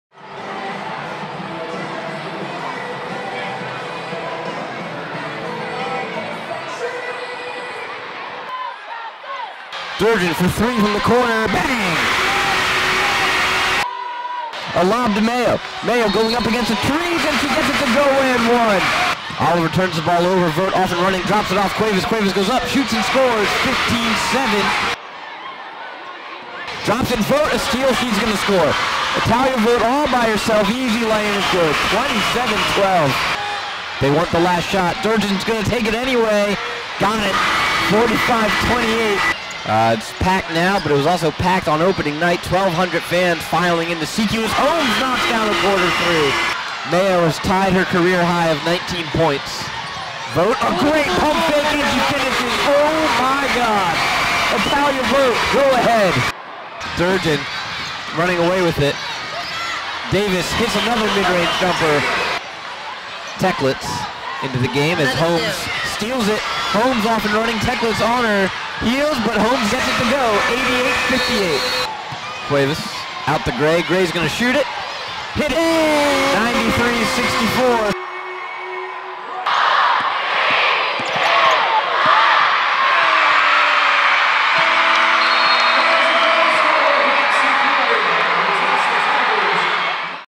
Towson Women's Basketball Gets First Win of 2017-2018 Season over UMBC
The Tigers were paced by career days by Nukiya Mayo, Danielle Durjan, and Etalyia Vogt.  This game marked the second-highest scoring game for the Tigers in program history.